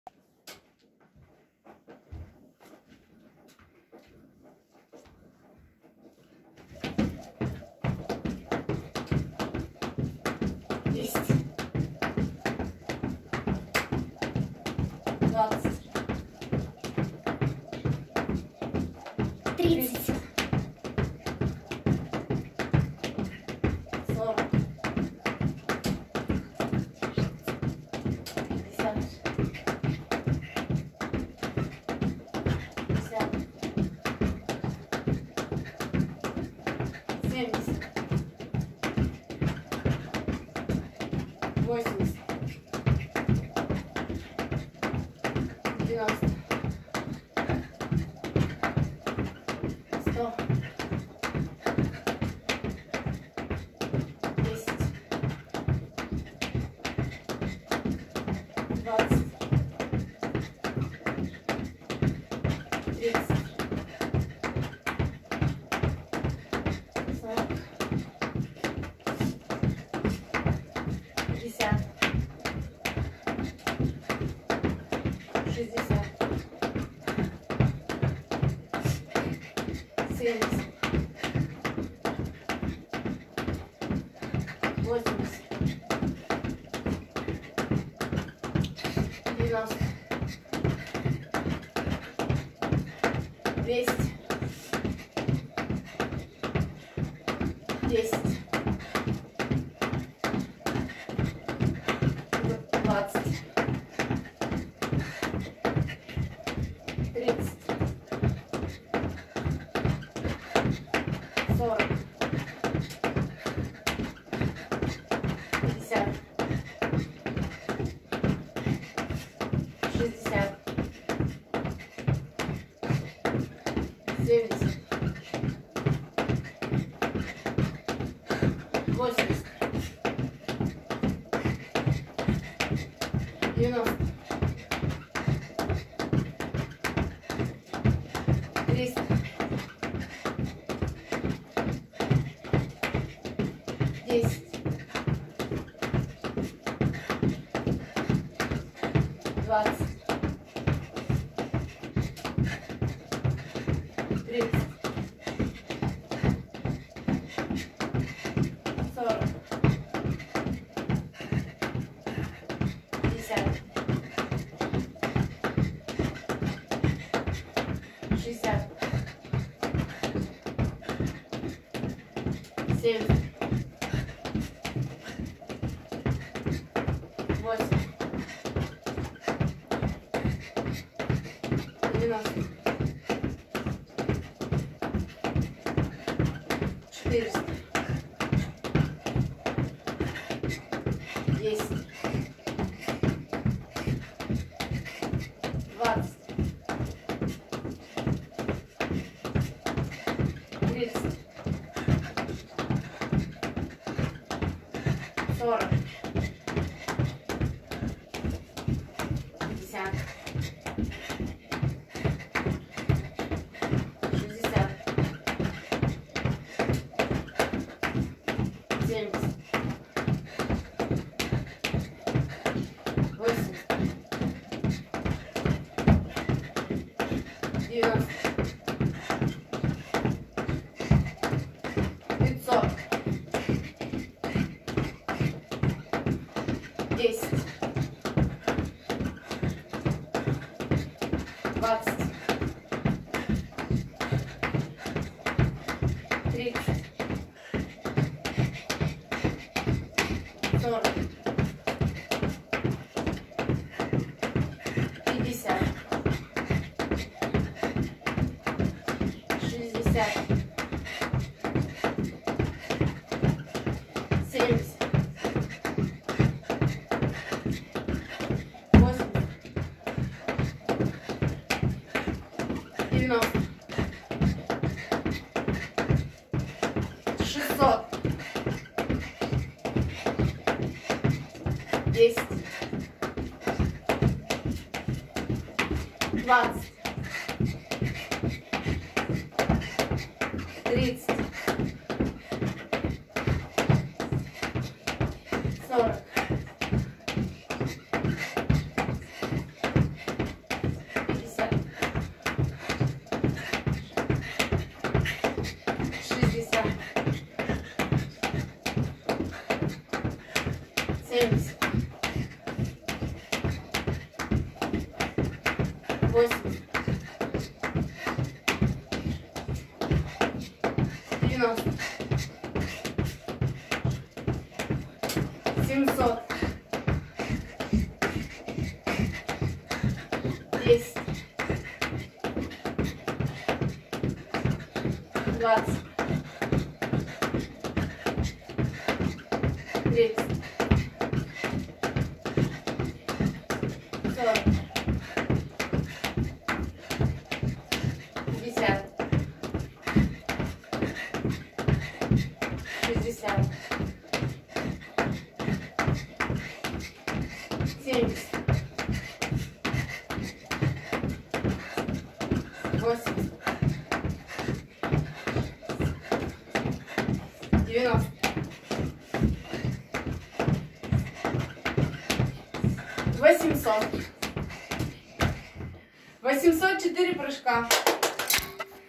Кузьмина Алиса, скакалка. 24.04.2020
Дата рождения - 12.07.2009. Возраст - 10 лет.
Для конкурса "Рекордсмены фитнес-аэробики Челябинской области!"